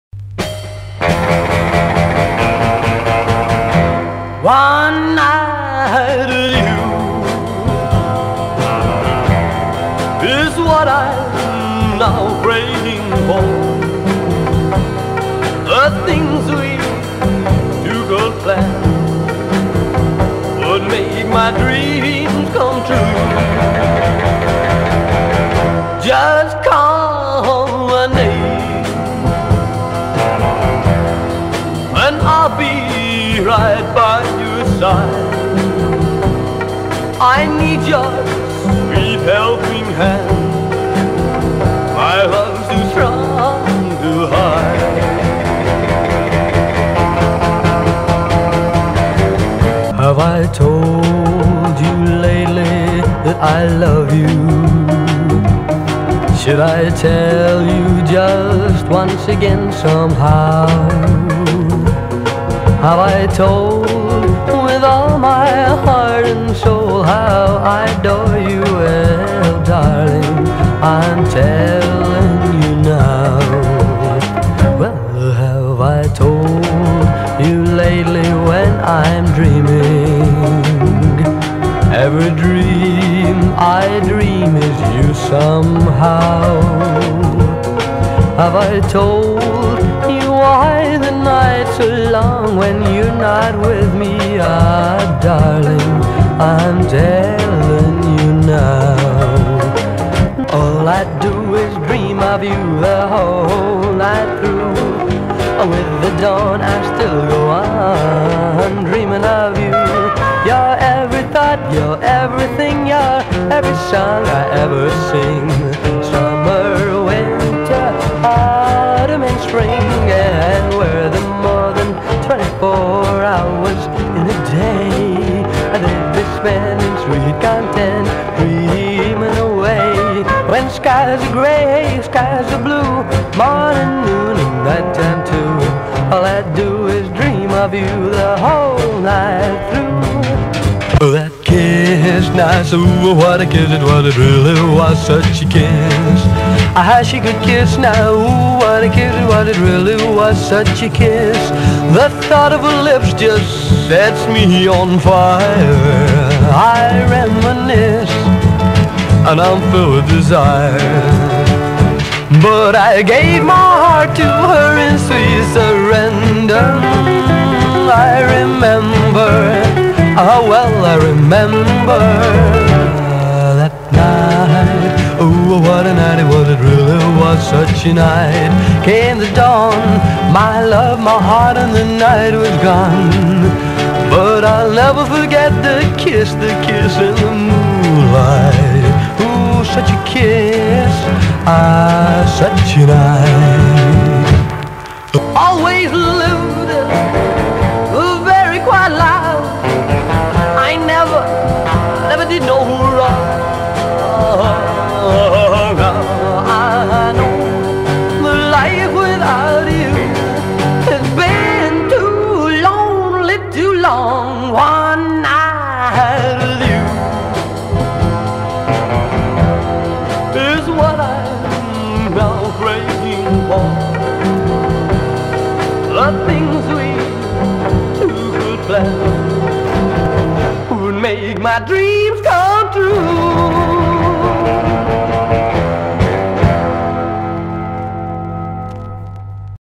Roy Dwain and The Roamers in Colour 1960-1963
Extracts from four songs recorded in the early 1960s by the Worksop group, with black and white photographs from the time all colorised in photoshop.